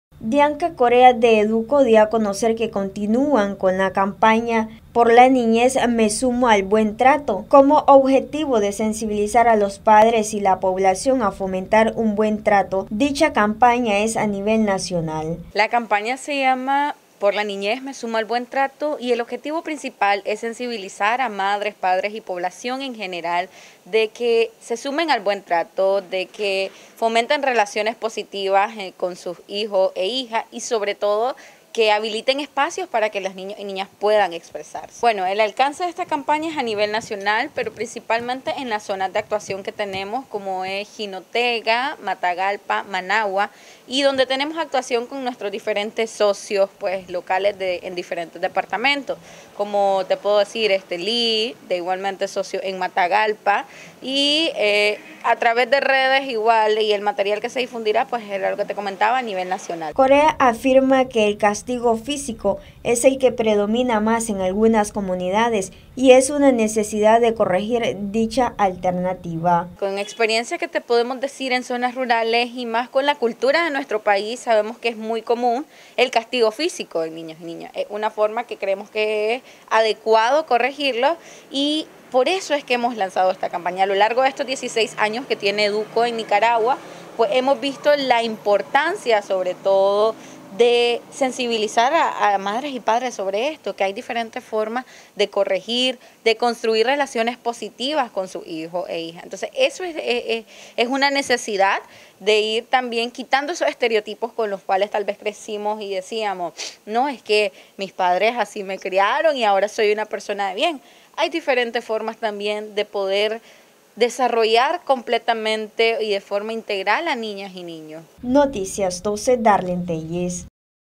Educa continúa impulsando la campaña“Por la Niñez me sumo al buen trato”
NosResistimosAlEmbargo Con el fin de continuar sensibilizando a los padres de familia y sociedad en general , Educo continúa impulsando la campaña “Por la Niñez me sumo al buen trato”.